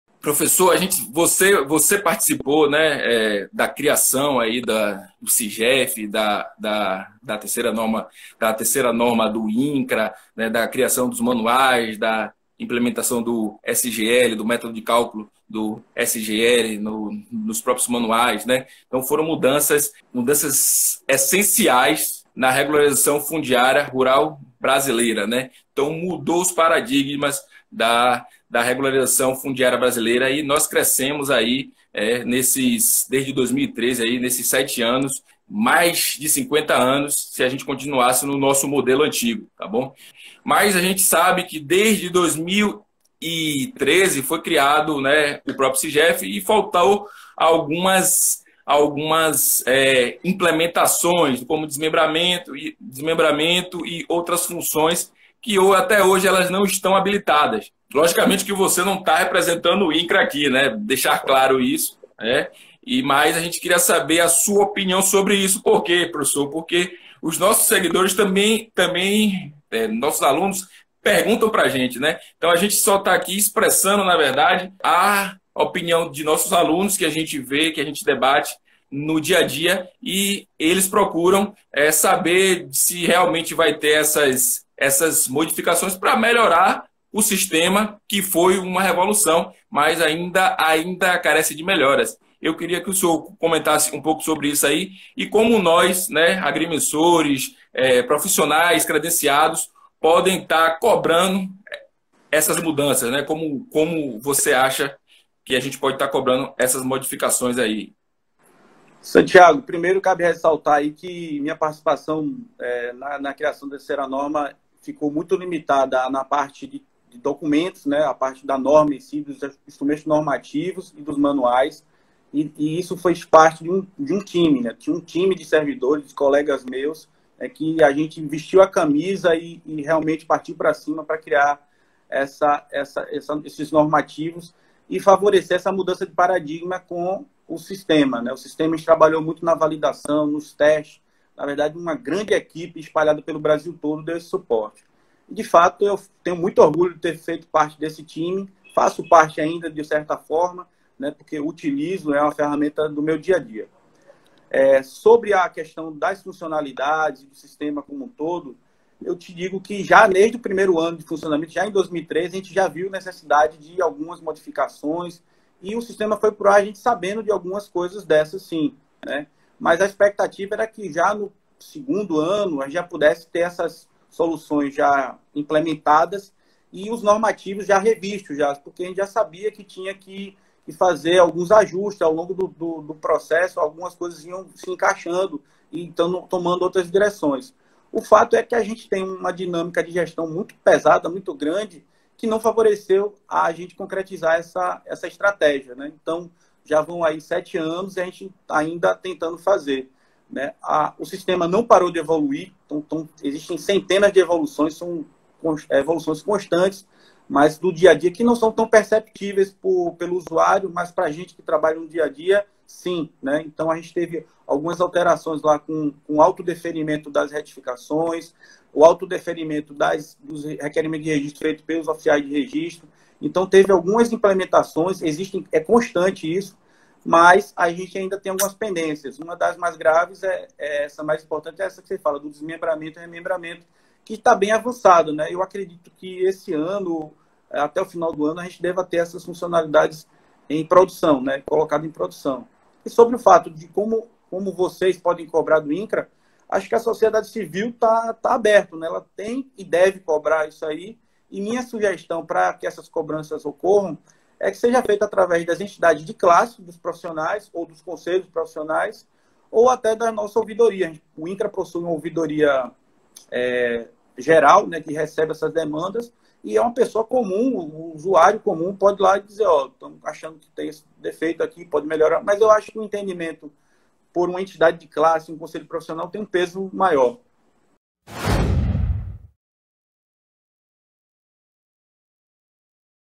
3º NORMA DO INCRA | CONVERSA COM O PROF. MIGUEL NETO
Em uma das nossas Lives lá no Instagram tivemos a participação do
Engenheiro e Professor Miguel Neto, trazendo informações essenciais
sobre assuntos polêmicos; e um deles foi sobre a 3º norma do INCRA.

Então acompanha ai e não esquece de deixar sua opinião
a respeito do tema.